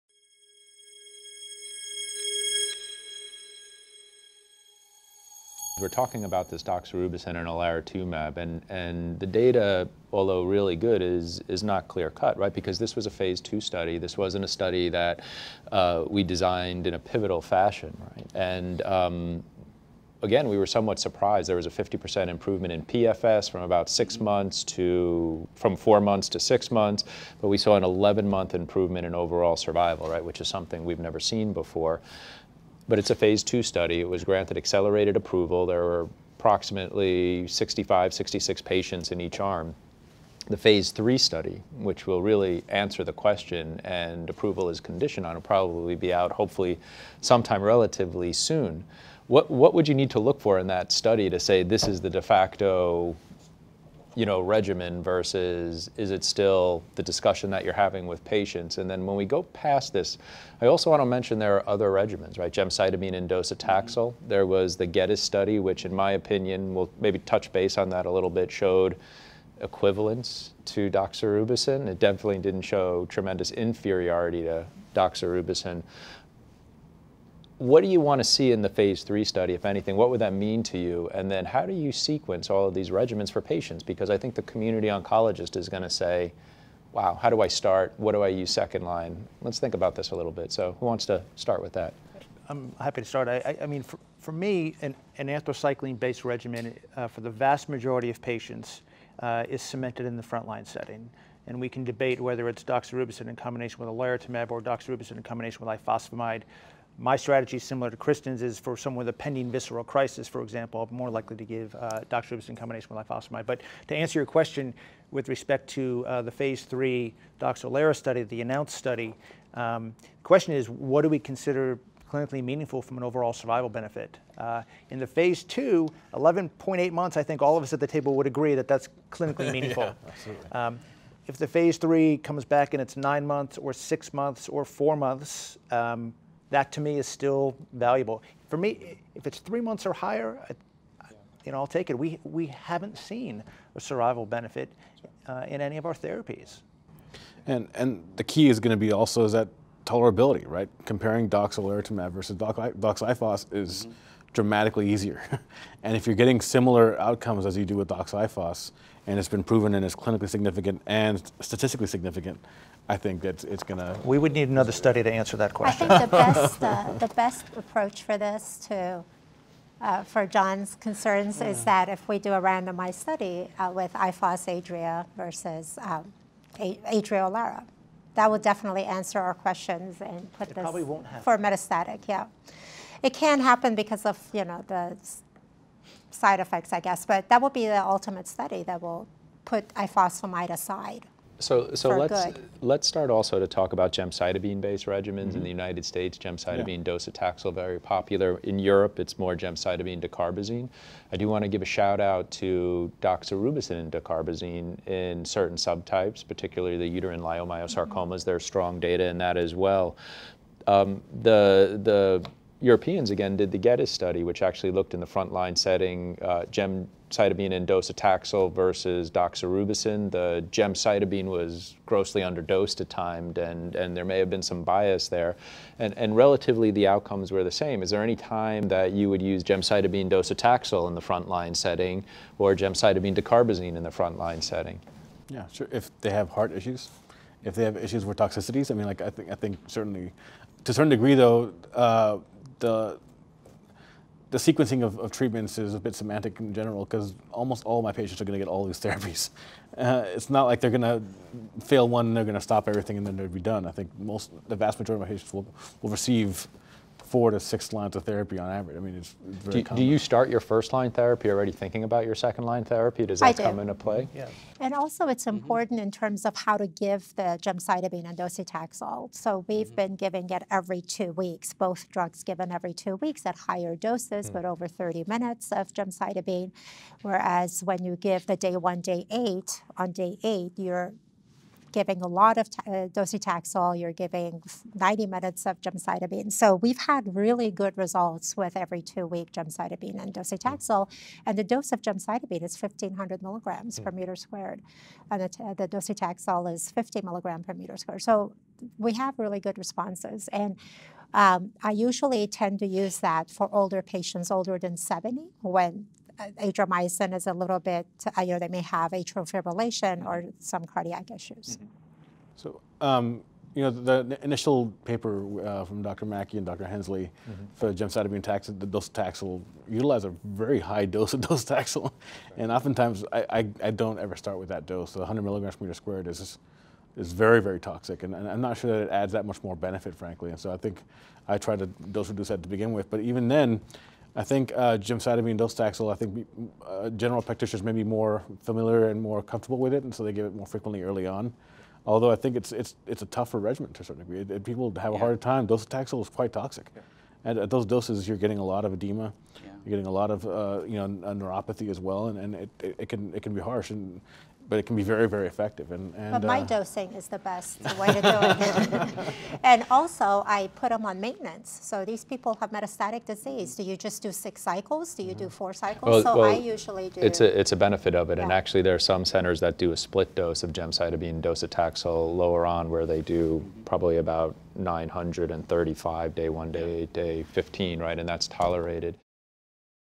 Olaratumab + Doxorubicin for Metastatic Soft-Tissue Sarcoma
William D. Tap, MD; Richard F. Riedel, MD; Victor M. Villalobos, MD, PhD; and Kristen N. Ganjoo, MD, comment on the overall survival benefit seen in the phase II study of doxorubicin and olaratumab, the phase III ANNOUNCE trial, and patient selection for this regimen and data for other regimens.